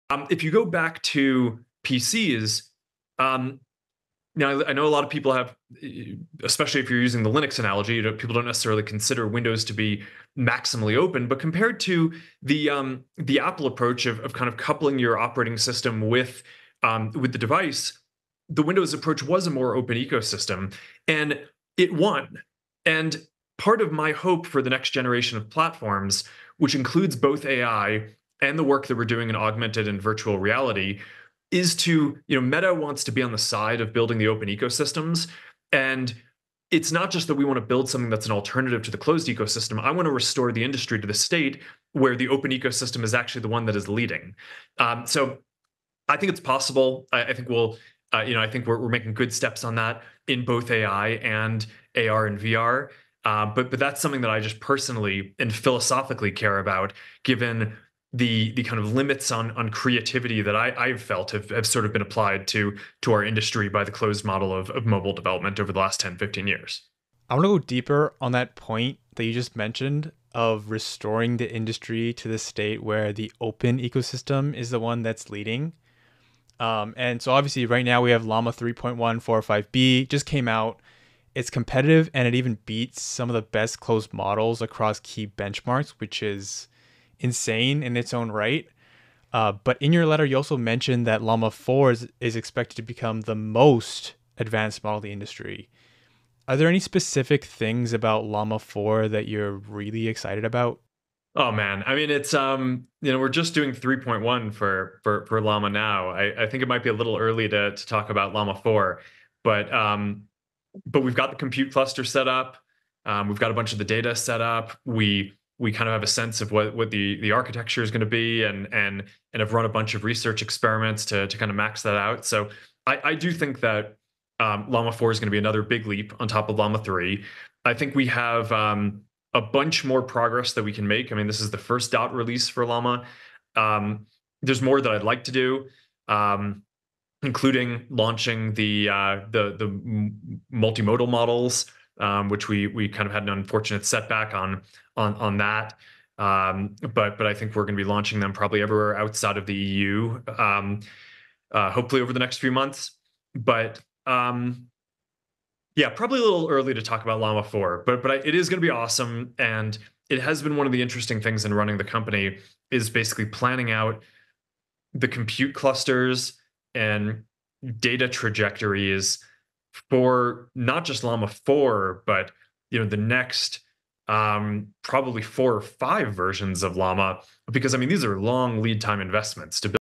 🌐 Mark Zuckerberg's Vision for Llama 4, 5, and 6
Need capital for your growing startup? Our mobile app is on the way.

In this exclusive clip, Mark Zuckerberg dives deep into the future of Meta’s AI models - Llama 4, 5, and beyond! Discover how Meta is pushing boundaries with these next-gen language models, aiming to make AI more powerful and accessible. 💡✨

👉 Will these advanced Llamas change the way we interact with technology? From personalized digital assistants to groundbreaking innovations in AI, Zuckerberg shares what’s next for AI’s evolution at Meta. Watch to find out what’s in store! 📲💥